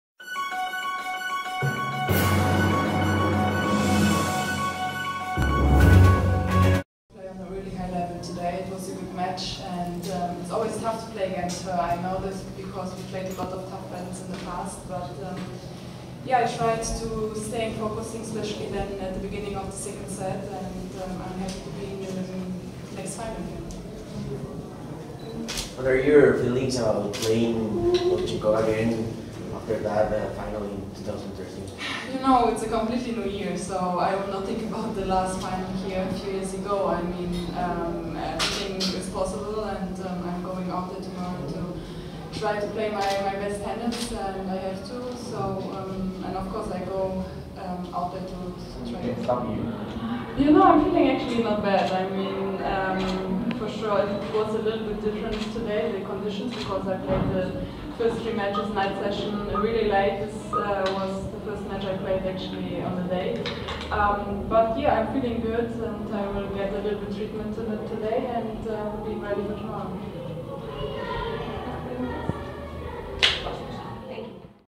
2017 Abierto GNP| Angelique Kerber Press Conference
Posterior al encuentro frente a Carla Suárez, Kerber compareció ante los medios para dar sus impresiones acerca de la final que disputará en el estadio GNP Seguros frente Anastasia Pavlyunchenkova.